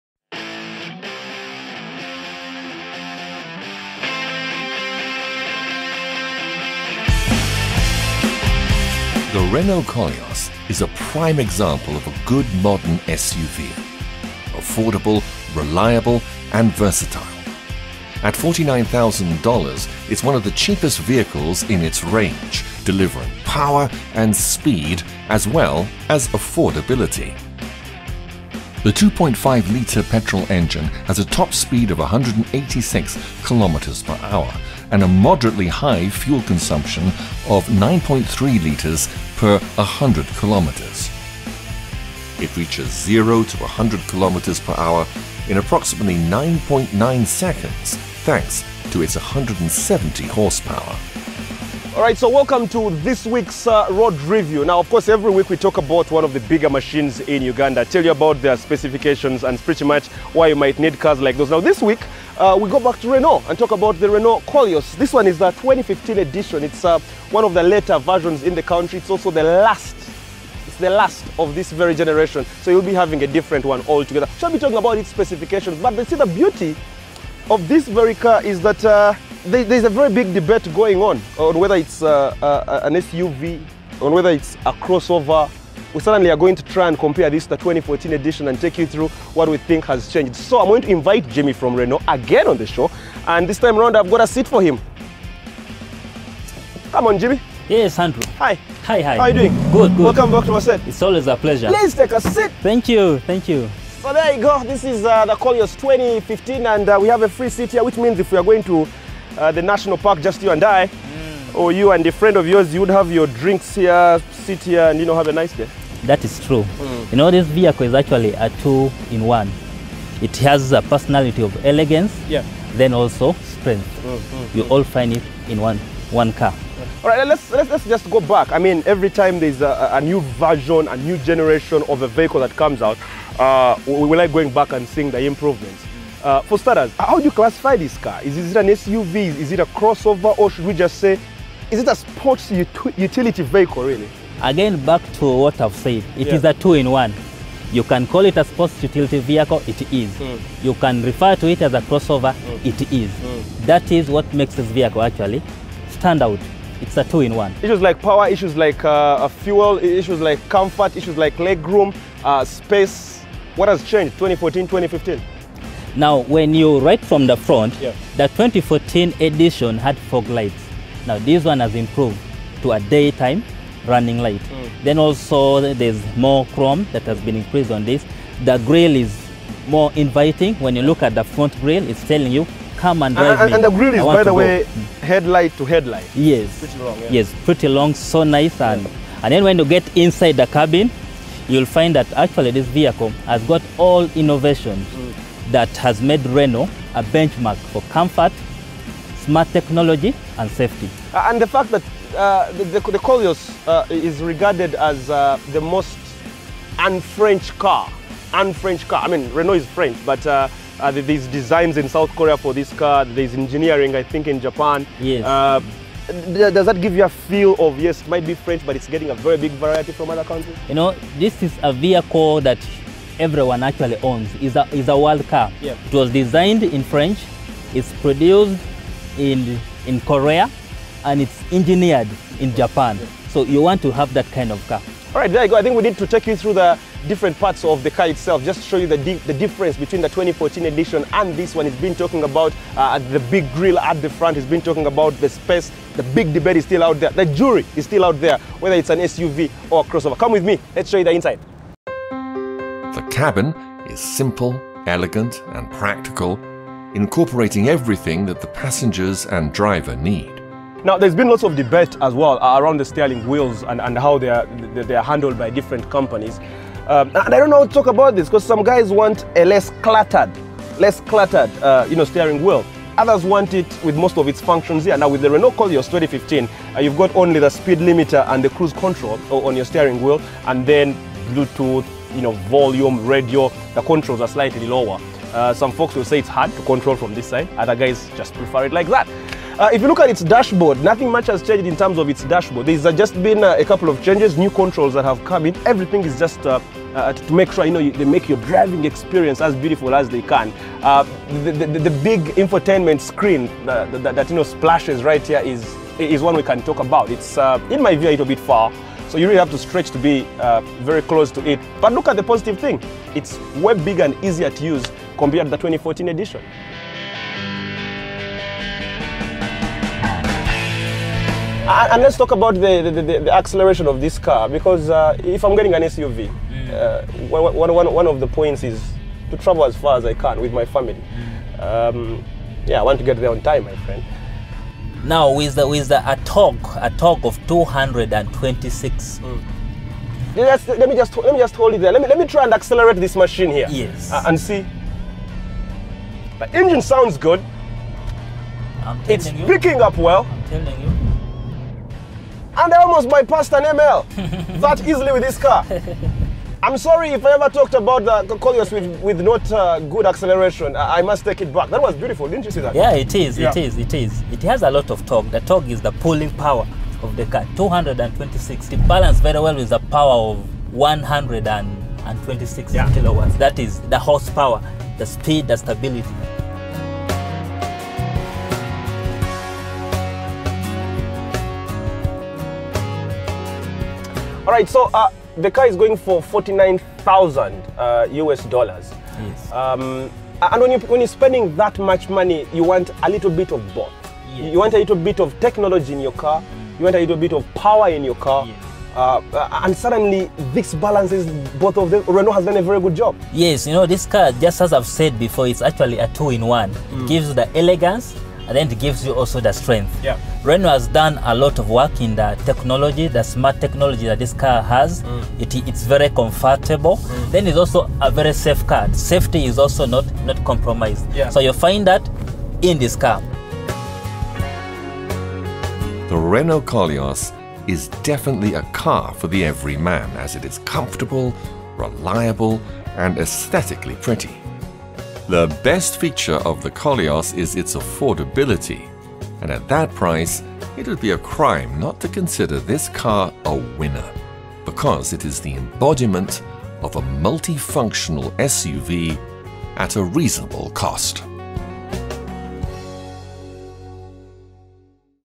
The Renault Koleos in Uganda - In Depth Review
The Renault Koleos is a prime example of a good modern SUV: affordable, reliable and versatile.